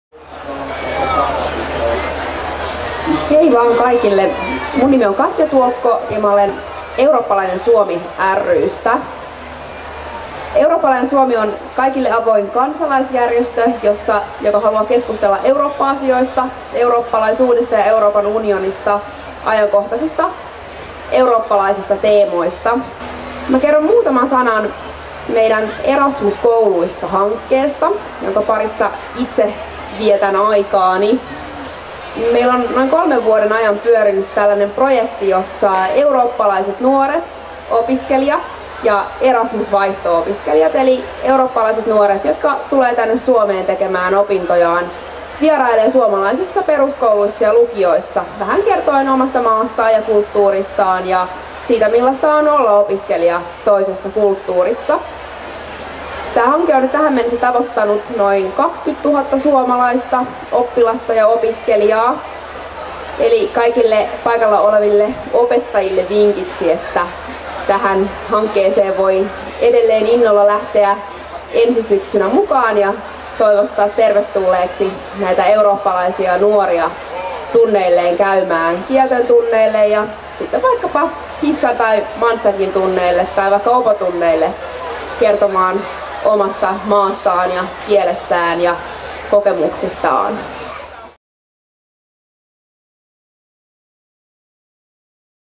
Erasmus kouluissa tuo kouluihin nuoria kertomaan omasta maastaan ja kulttuurista
Eurooppalainen Suomi ry:n Katja Tuokko kertoi Maailma kylässä -festarilla tosi kompaktisti Erasmus kouluissa -hankkeesta. Erasmus kouluissa on tavoittanut jo 20 000 oppilasta ja opiskelijaa. Peruskoulujen ja lukioiden oppilailla ja opettajilla on mahdollisuus kutsua vieraaksi eurooppalaisia vaihto-opiskelijoita, jotka saapuvat kertomaan kotimaastaan ja kulttuuristaan.